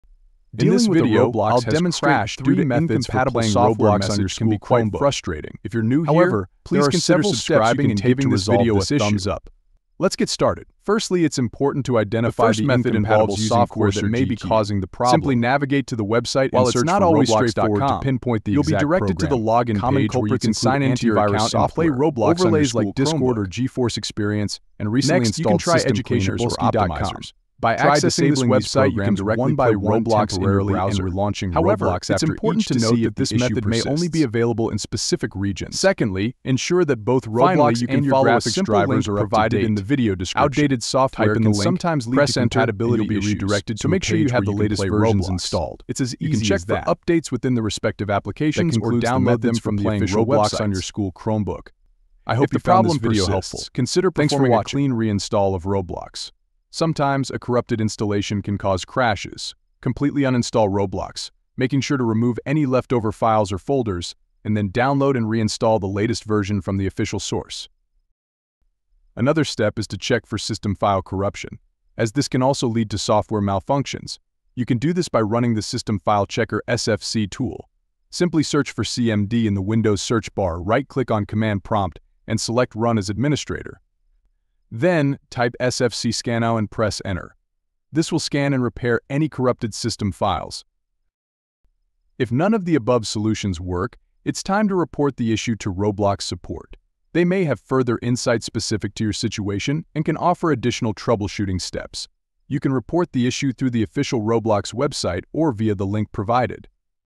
Roblox Has Crashed Due To Incompatible Software | FIX
In this video I show you How to fix Roblox Has Crashed Due to Incompatible Software.